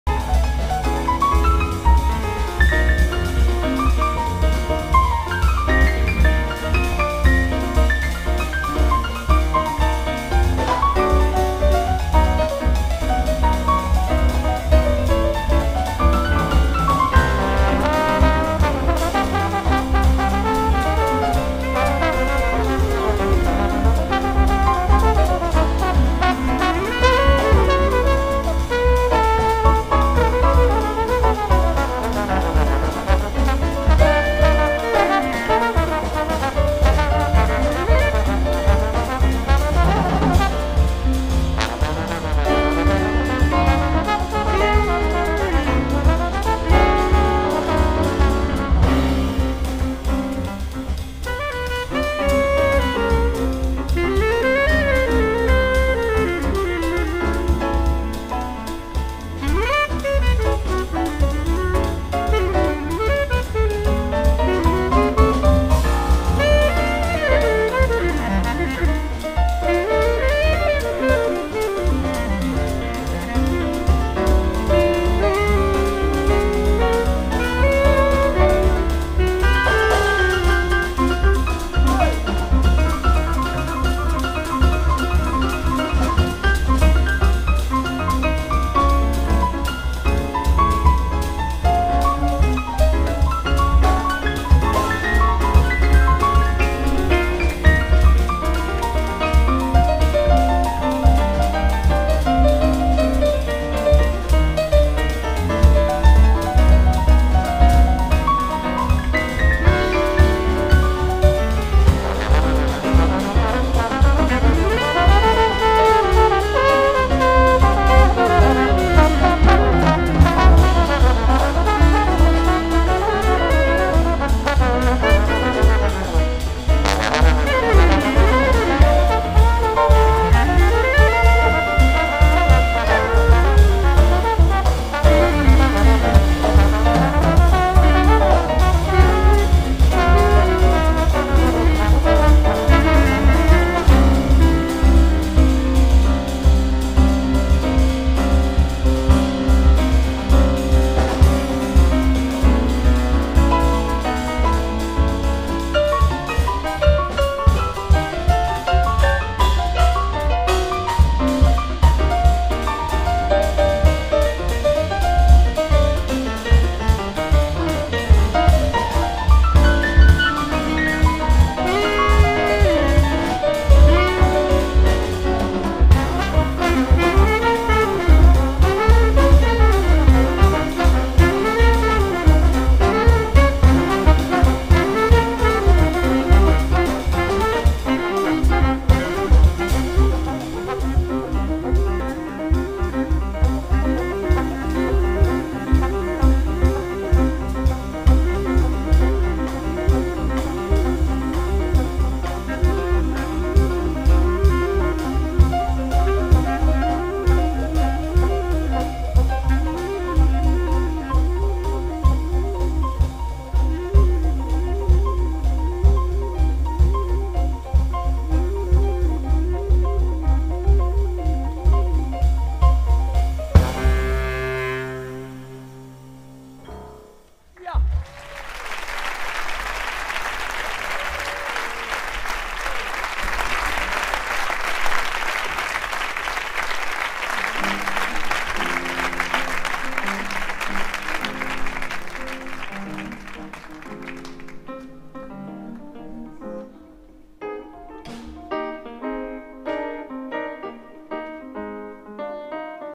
55 Jazzaldia: concierto de Michel Portal New Quintet (2020)
Concierto de Michel Portal New Quintet  Kontzertua - 55 Jazzaldia. Donostia / San Sebastián, Plaza de la Trinidad/Trinitate Plaza  23 /07/2020

# Michel Portal Bojan Z. Bruno Chevillon Nils Wogram Jeff Ballard